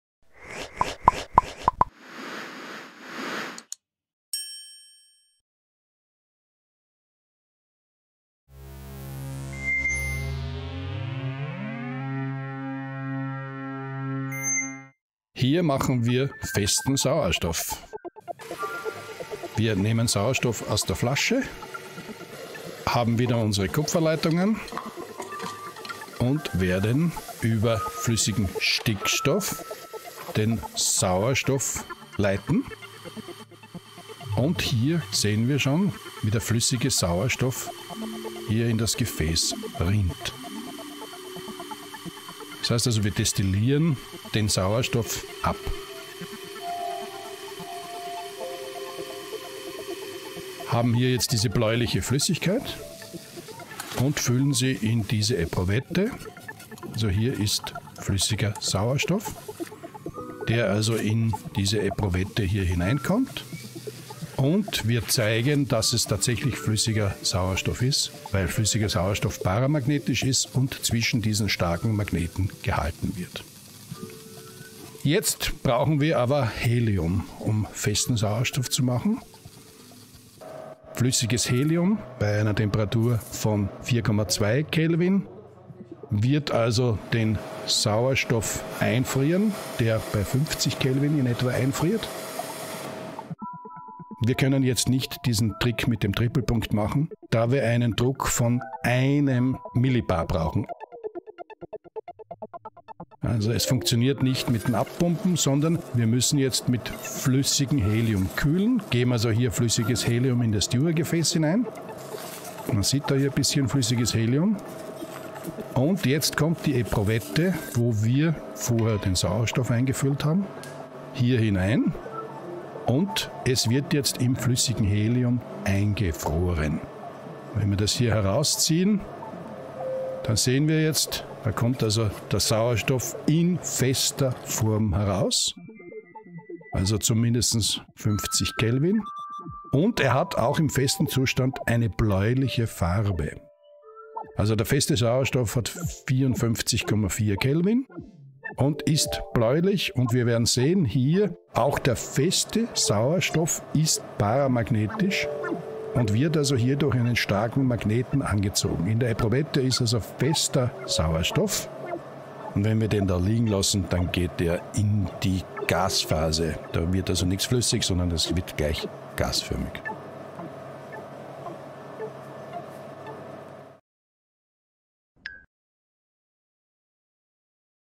Mit diesem Trick festen Sauerstoff herstellen: Einfrieren in flüssigem Helium
Dies ist ein Video des Kurses "Aha-Erlebnisse aus der Experimentalphysik Teil 2"

Vakuumpumpen Hersteller hassen diesen Trick: Prof. Pottlacher zeigt in diesem Video,
wie man ohne Vakuumpumpe festen Sauerstoff herstellen kann.

Als erstes wird gezeigt, wie man gasförmigen Sauerstoff in flüssigem Stickstoff destilliert. Der daraus resultierende flüssige Sauerstoff hat bereits eine bläuliche Farbe und ist paramagnetisch.

Für die Herstellung von festem Sauerstoff bedient man sich entweder extrem niedrigen Druck oder extrem niedriger Temperaturen. Um mittels Druckabsenkung festen Sauerstoff herzustellen, benötigt man eine Spezialpumpe, welche in der Lage ist den umliegenden Druck auf 0.0015 bar abzusenken. Alternativ dazu kann man festen Sauerstoff auch durch Einfrieren in flüssigem Helium herstellen. Mit 4,2 Kelvin oder -269°Celsius hat Helium den niedrigsten Siedepunkt aller Gase, weshalb flüssiges Helium der kälteste Stoff auf der Erde ist.

Echter fester Sauerstoff hat dann auch wieder eine bläuliche Farbe und ist wiederum paramagnetisch.

Vielen Dank an Prof. Knoll, Prof. Heinz Krenn und Jacob Reynvaan für das Ermöglichen dieses Experiments an der KFU Graz.